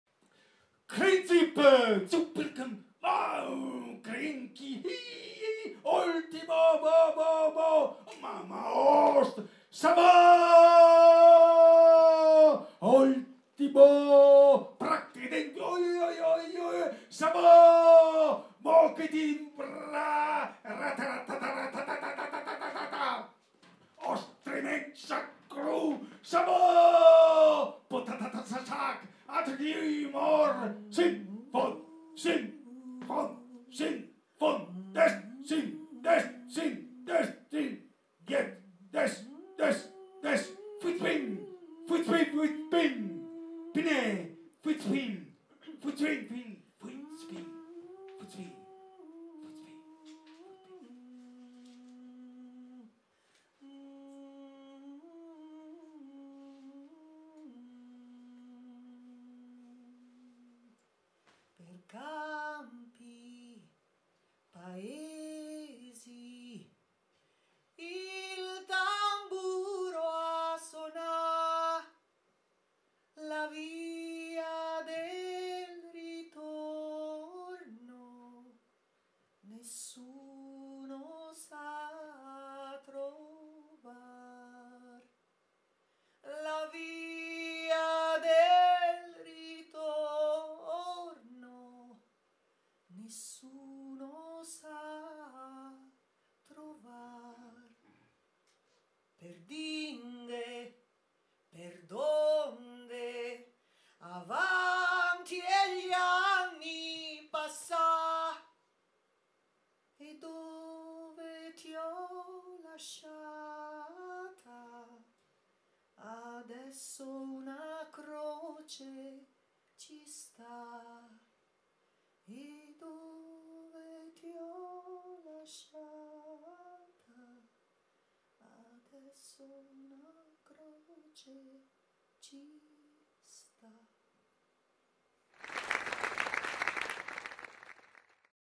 DEPEROFONÌE: con LAURA FACCI & MAURO DAL FIOR
ALLA BAJONETTA di Fortunato Depero (con canzone della Grande Guerra)
       dallo spettacolo:
       PAROLE IN LIBERTA' A DUE PER FORTUNATO DEPERO
      Teatro MODUS -Piazza Orti di Spagna (San Zeno) Verona
       30 marzo 2023